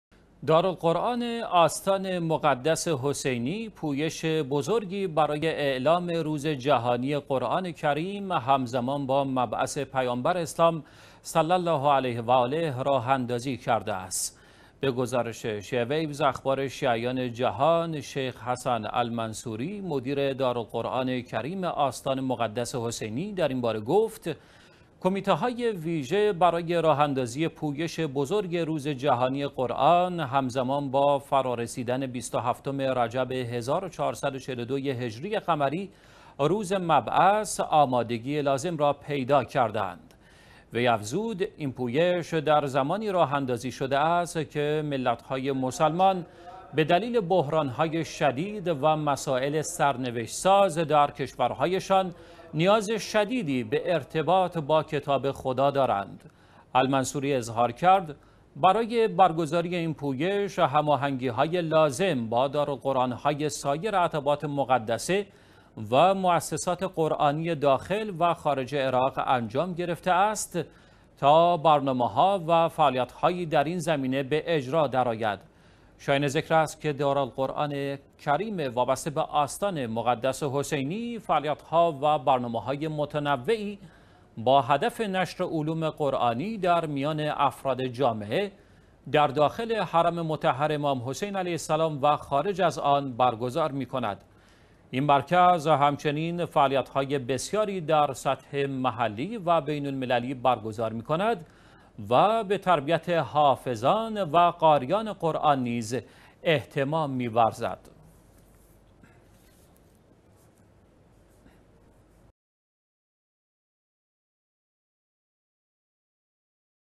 اعلام روز جهانی قرآن کریم هم‌زمان با مبعث پیامبر اسلام
دارالقرآن آستان مقدس حسینی پویش بزرگی برای اعلام روز جهانی قرآن کریم هم‌زمان با مبعث پیامبر  اسلام صلی الله علیه و آله راه‌اندازی کرده است.
به گزارش شیعه ویوز (اخبار شیعیان جهان)؛ شیخ "حسن المنصوری" مدیر دارالقرآن الکریم آستان مقدس حسینی در این باره گفت: کمیته‌های ویژه برای راه‌اندازی پویش بزرگ روز جهانی قرآن هم‌زمان با فرا رسیدن ۲۷ رجب ۱۴۴۲ هجری قمری (روز مبعث) آمادگی لازم را پیدا کرده‌اند.
وی افزود: این پویش در زمانی راه‌اندازی شده است که ملت‌های مسلمان به دلیل بحران‌های شدید و مسائل سرنوشت‌ساز در کشورهایشان، نیاز شدیدی به ارتباط با کتاب خدا دارند.
المنصوری اظهار کرد: برای برگزاری این پویش هماهنگی‌های لازم با دارالقرآن‌های سایر عتبات مقدسه و مؤسسات قرآنی داخل و خارج عراق انجام گرفته است تا برنامه‌ها و فعالیت‌هایی در این زمینه به اجرا درآید.
شایان ذکر است که دارالقرآن الکریم وابسته به آستان مقدس حسینی فعالیت‌ها و برنامه‌های متنوعی با هدف نشر علوم قرآنی در میان افراد جامعه، در داخل حرم مطهر امام حسین علیه السلام و خارج از آن برگزار می‌کند.
این مرکز همچنین فعالیت‌های بسیاری در سطح محلی و بین‌المللی برگزار می‌کند و به تربیت حافظان و قاریان قرآن نیز اهتمام می‌ورزد.